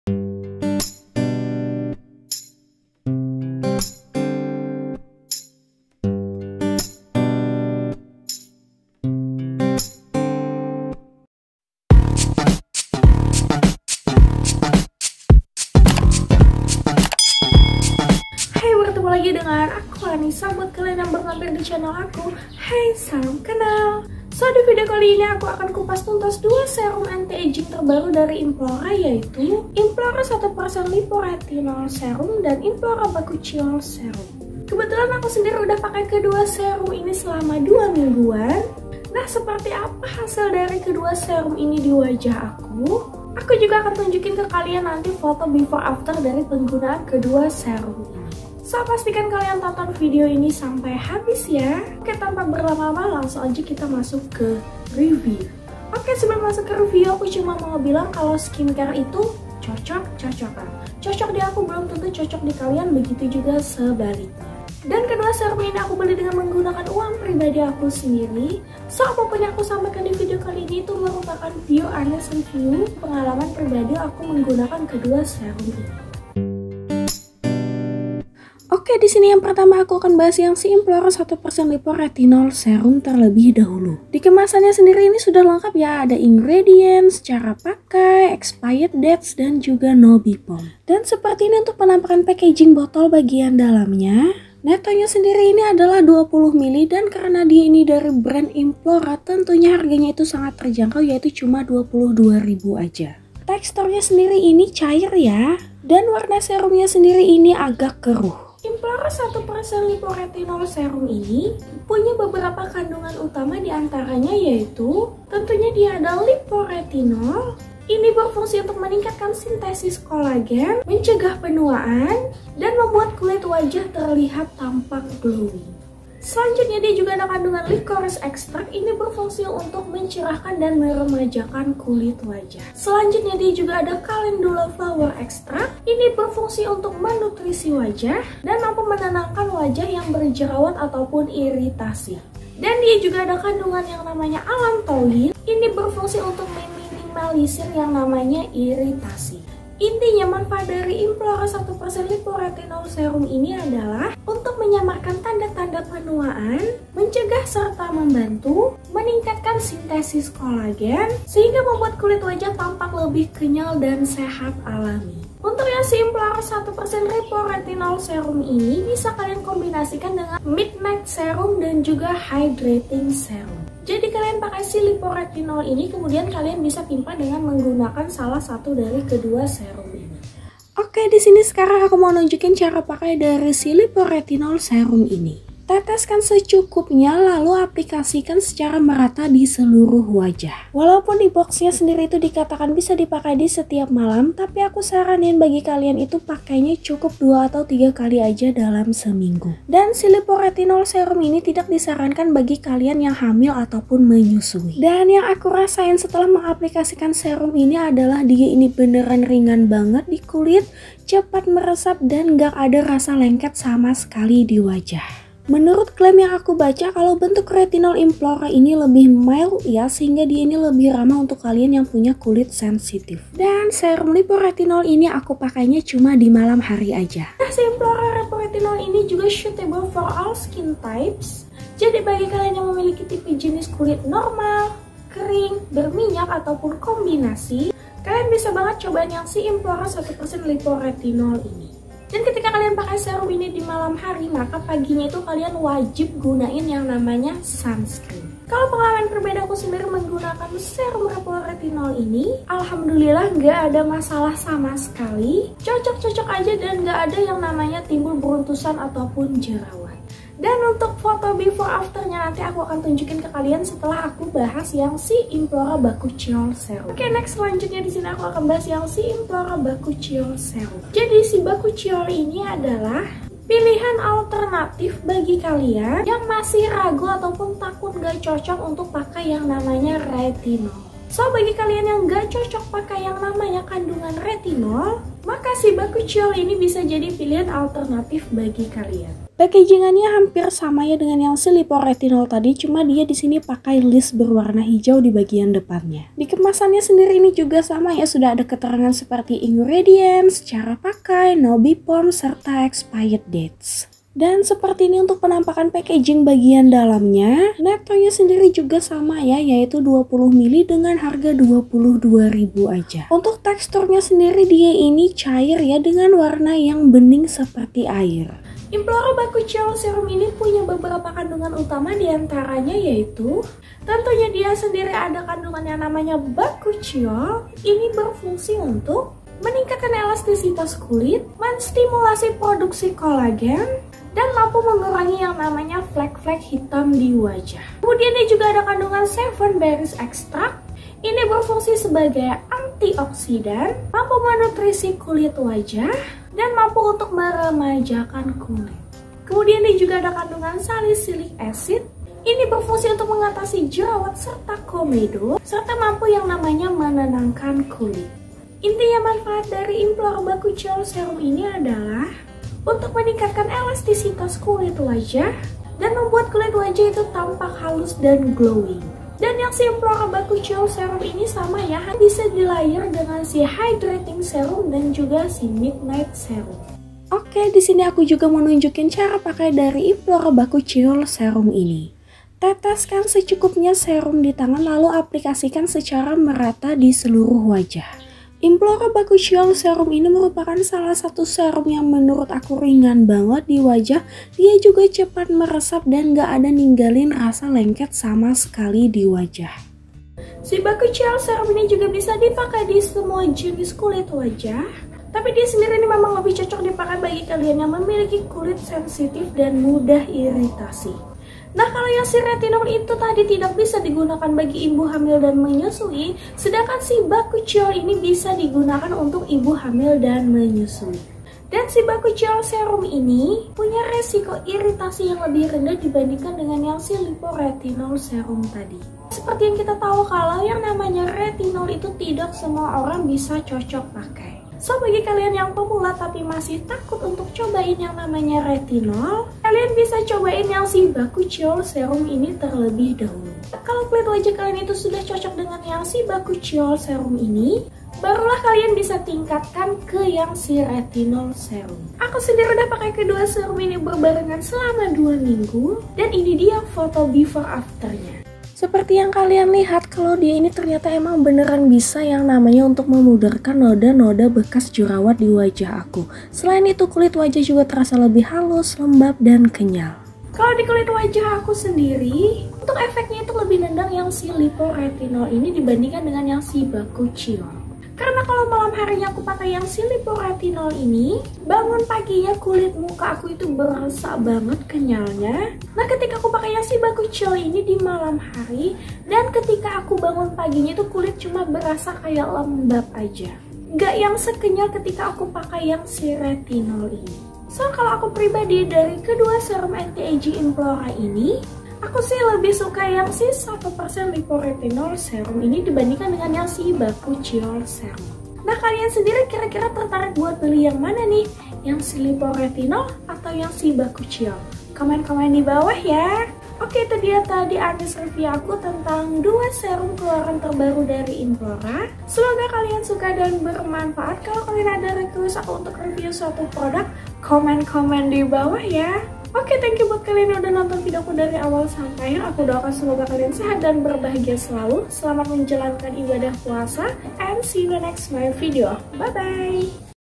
BATTLE IMPLORA RETINOL SERUM & IMPLORA BAKUCHIOL SERUM
Hai kali ini aku mau review sekaligus batte dua serum anti aging keluaran terbaru dari implora yaitu implora 1% liporetinol serum dan implora bakuchiol serum. Kira-kira lebih worth to buy yang mana nih? simak divideo yaaa